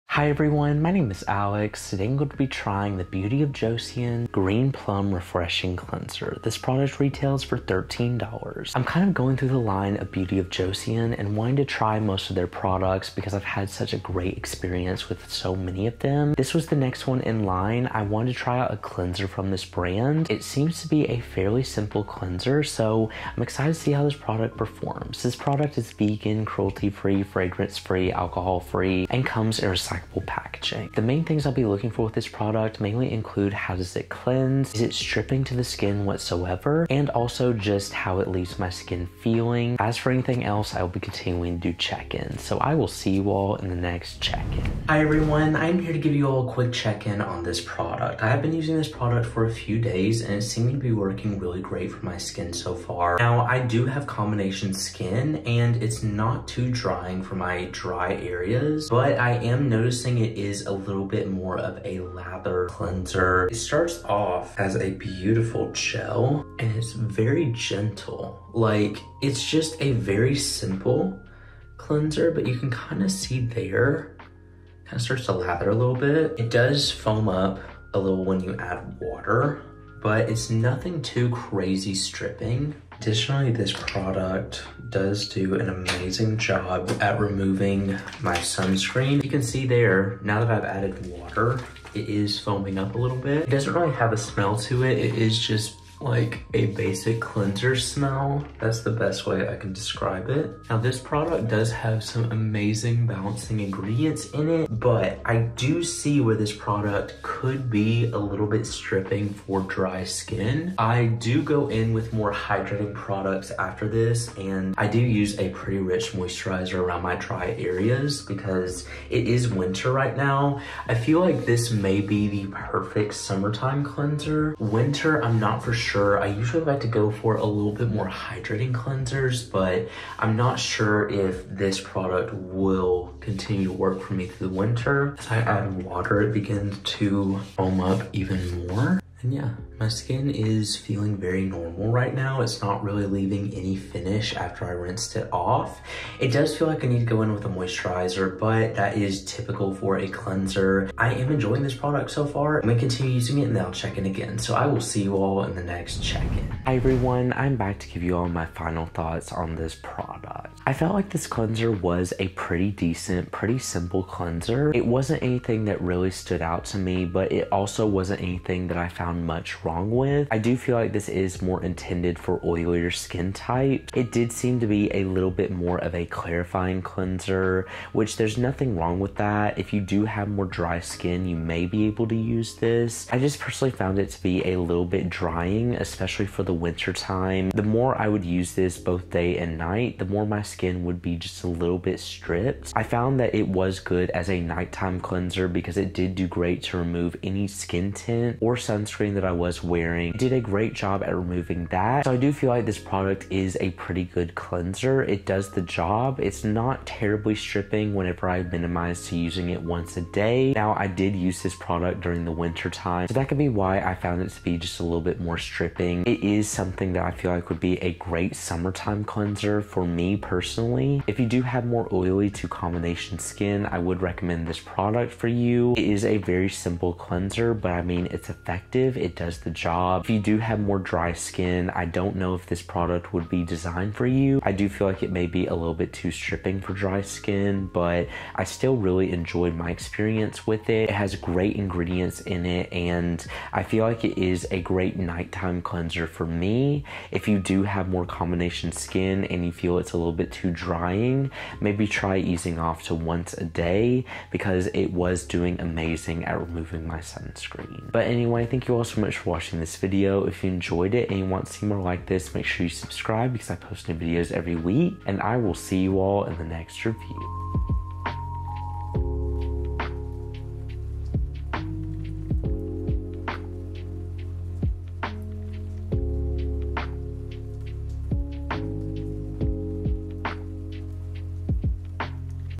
beauty of joseon green plum refreshing cleanser | trial + review
hello everyone 👋🏻

today i am trying and reviewing the beauty of joseon green plum refreshing cleanser

please note that this trial was conducted within a span of 1-2 weeks so there are no long-term skin impacts shown in this video.

00:00 introduction
00:25 product information
00:44 using the product
02:36 final thoughts